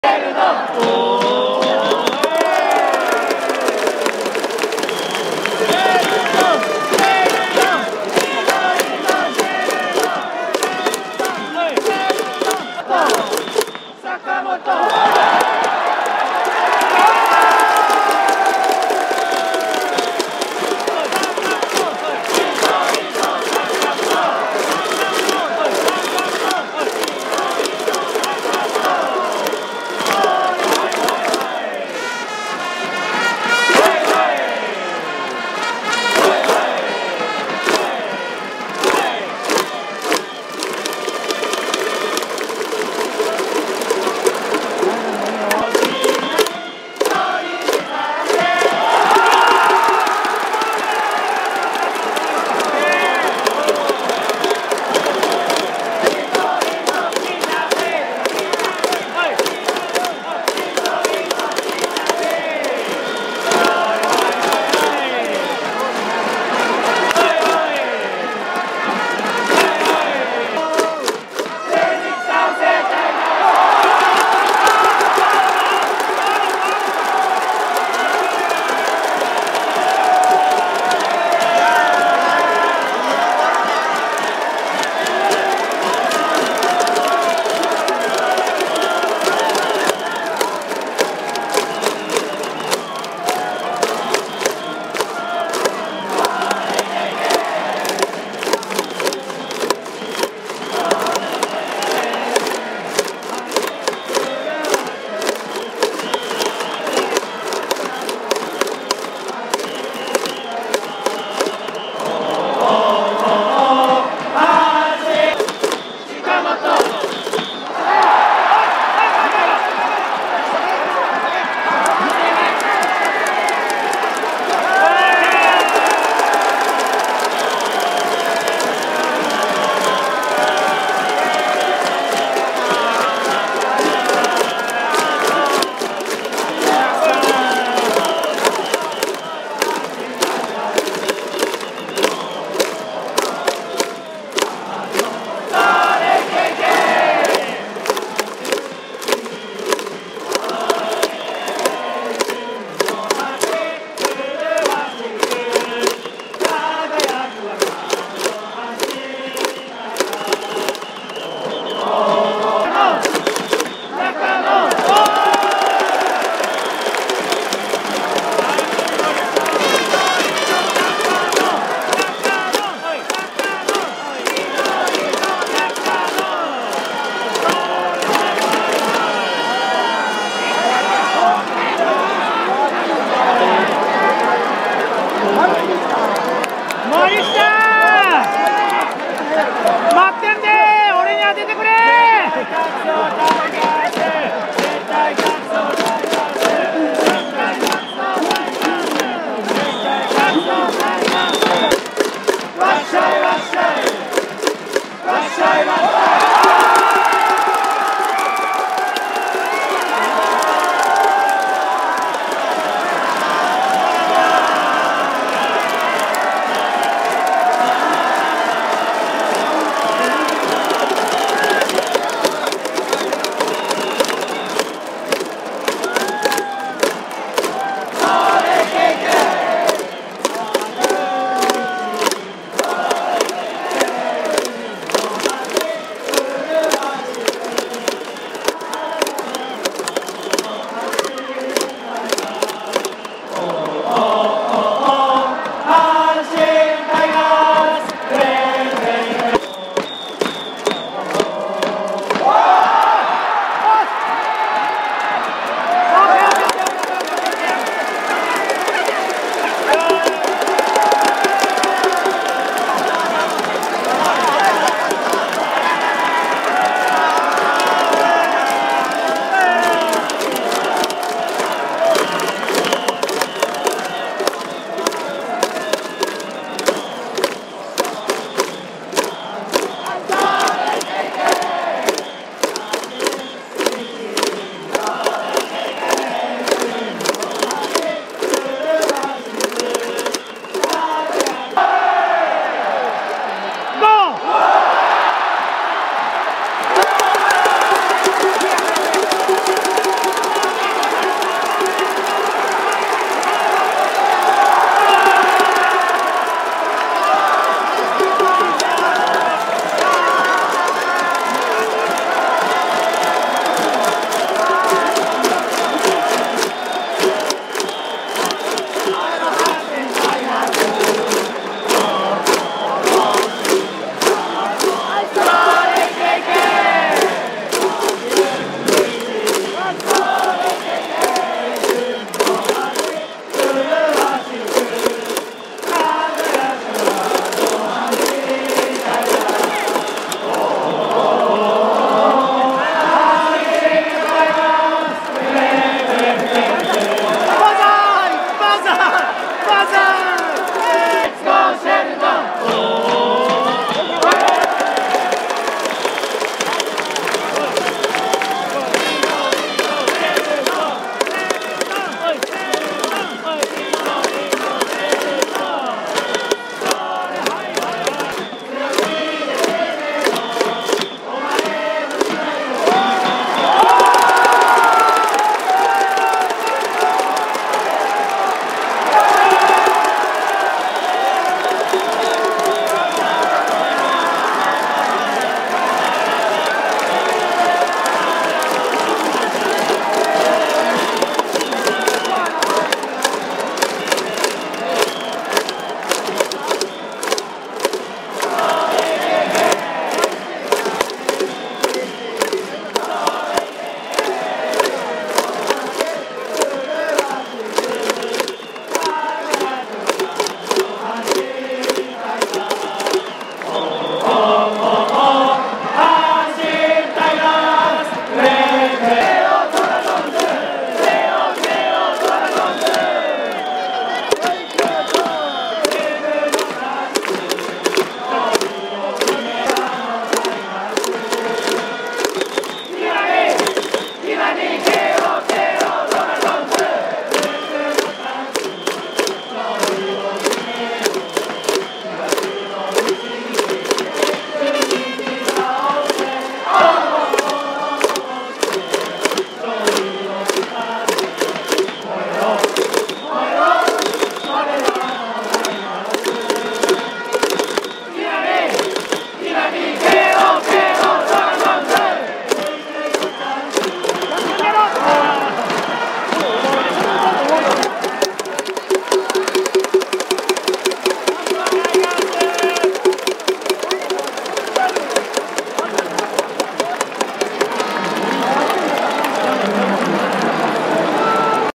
【2回裏ハイライト】猛虎打線大爆発！打者一巡の猛攻で一挙7得点のビッグイニング！ 2024.4.20
2024年4月20日 阪神タイガース対中日ドラゴンズ in 甲子園球場